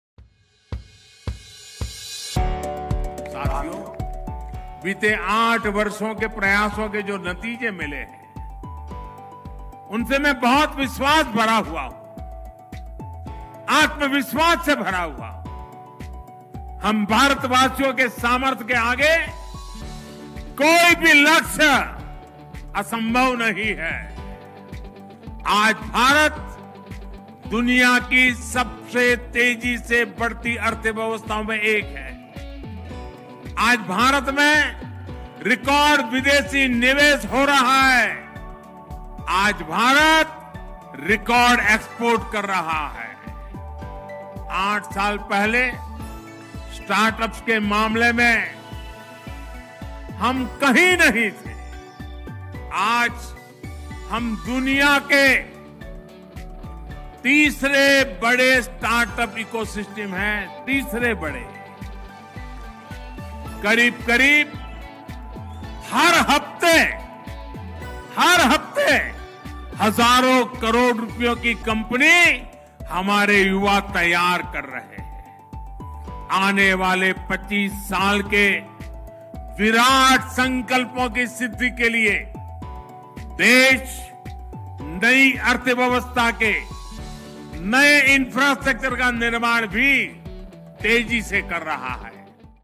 हम भारत वासियों के सामर्थ्य के आगे कोई भी लक्ष्य असंभव नहीं है
आज भारत दुनिया की सबसे तेजी से बढ़ती अर्थव्यवस्थाओं में से एक है।

आज भारत में रिकॉर्ड विदेशी निवेश हो रहा है। आज भारत रिकॉर्ड एक्सपोर्ट कर रहा है